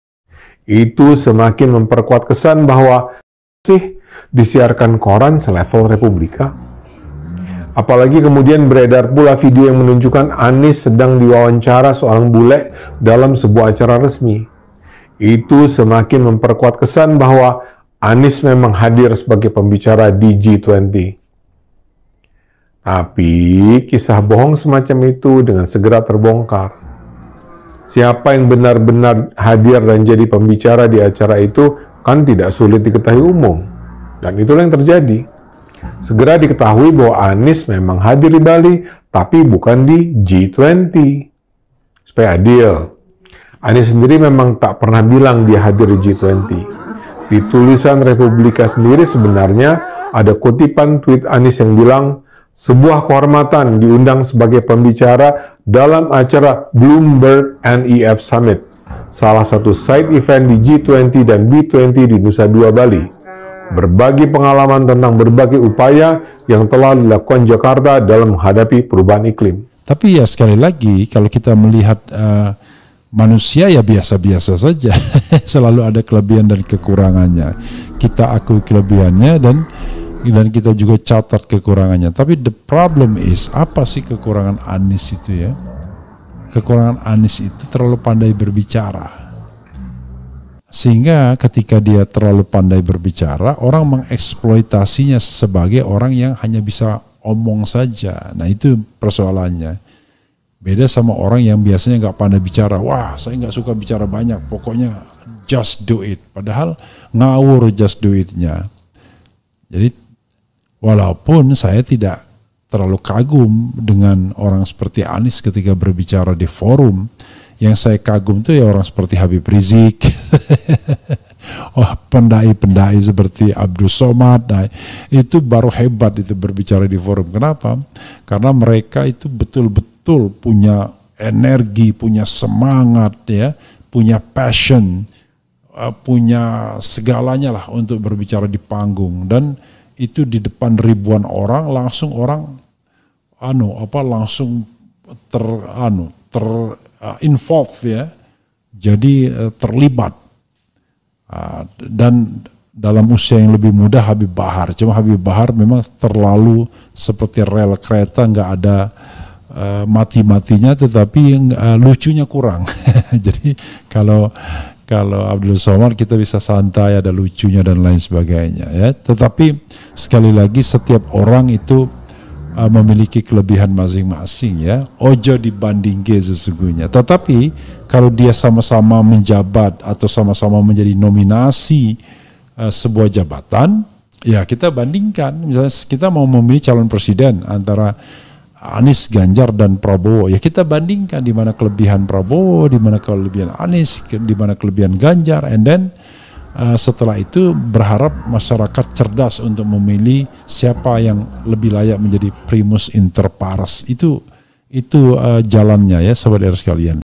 BERITA VIRAL~ ANIS BASWEDAN MENEMUI PRESIDEN AS, URAT MALU PENDUKUNG ANIS SUDAH PUTUS
kalo mau menang jadi presiden tahun 2024, jangan banyak berbohong hanya karena ingin popularitas, ini jaman sekarang sudah berubah, gk akan ada yang bisa di bohongi lagi.

illustratsi thumbnail !!!

berita viral, berita terkini, berita terbaru, berita hari ini
■■■■■■■■■■■■■o■■■■■